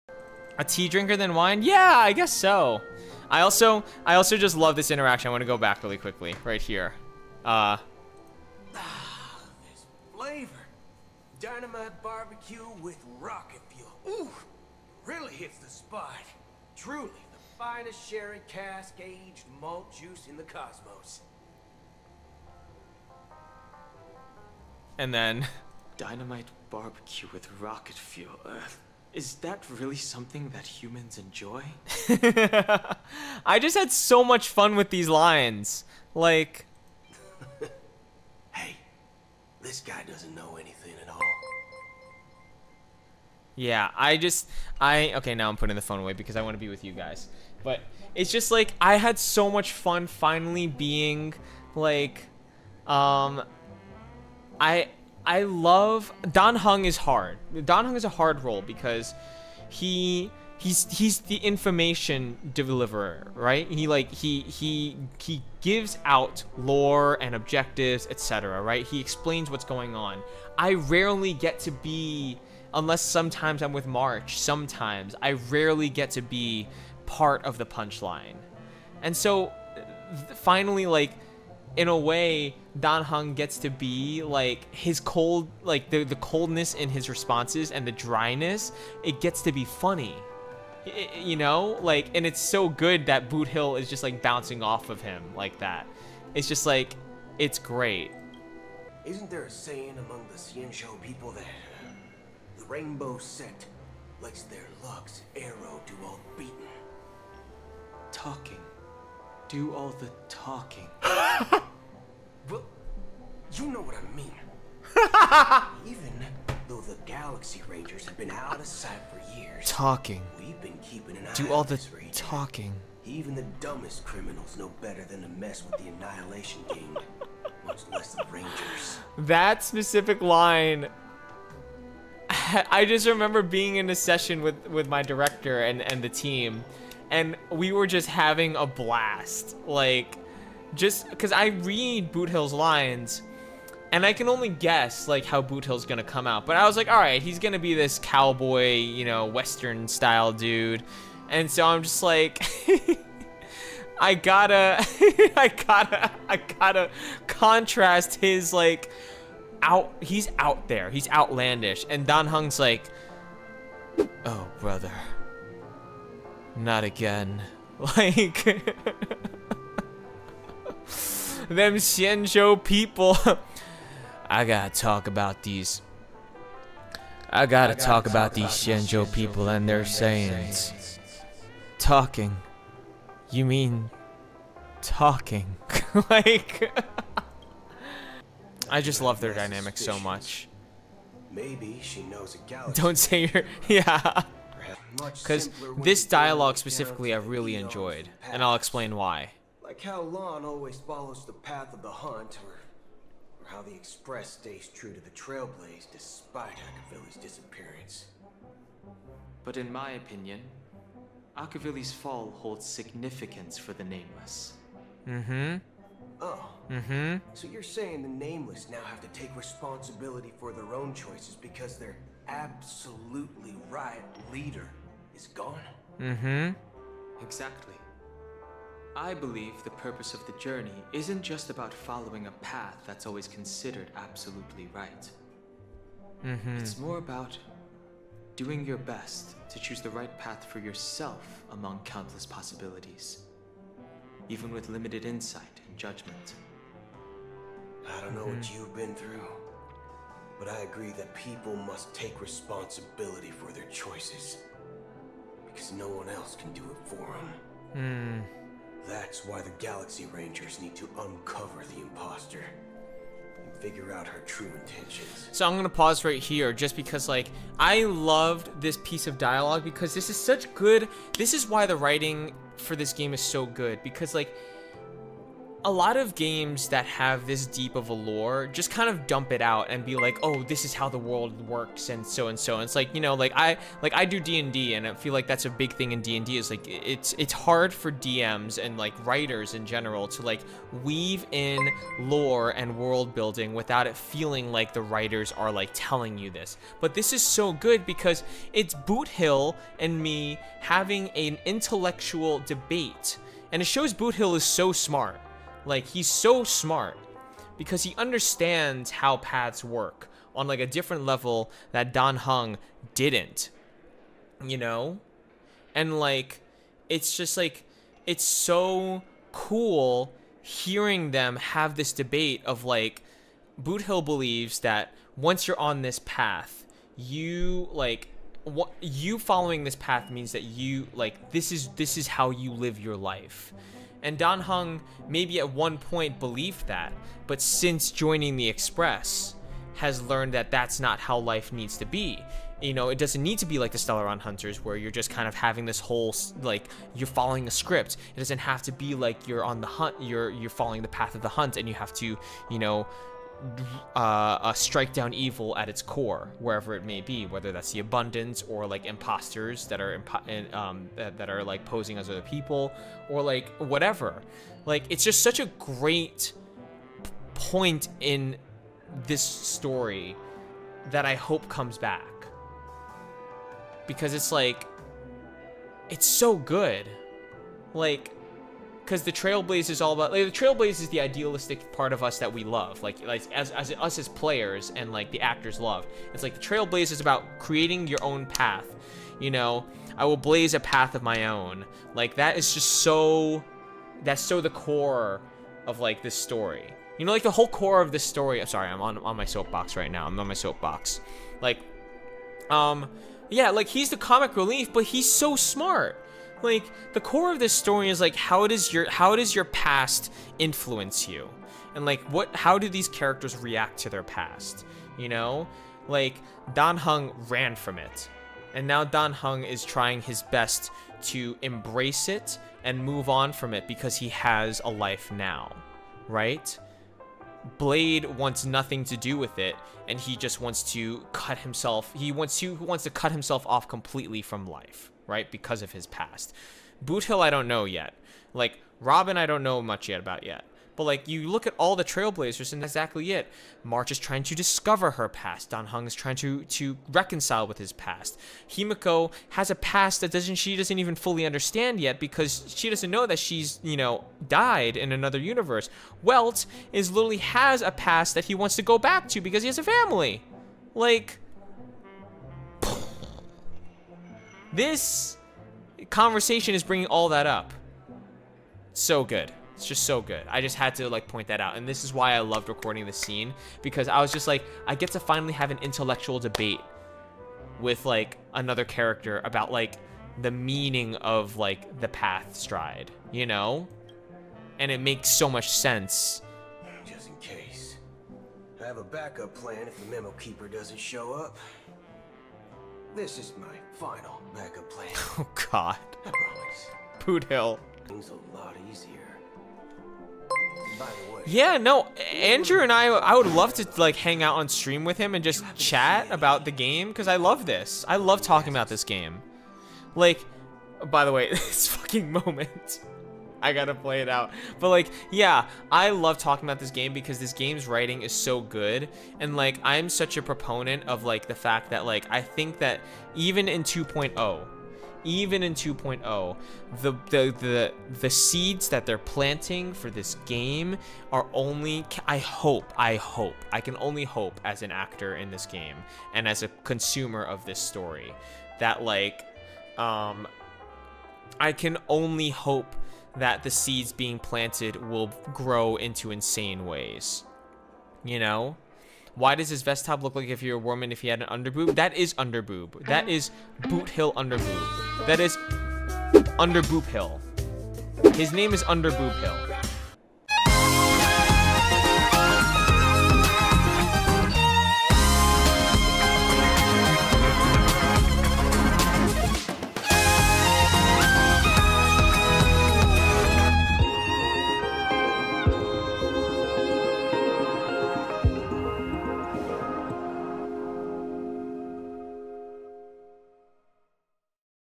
Dan Heng EN VA Talks About The Recording Experience of His and Boothill Scenes and Their Dynamics
Honkai: Star Rail Dan Heng's English Voice, Mr. Nicholas shares a moment of his experience of recording one of his character's interaction with Boothill and talks about the dynamics of the duo. Did minimal editing to keep ingenuity.
As usual, his related links will be in the description. Let's show him some love there too!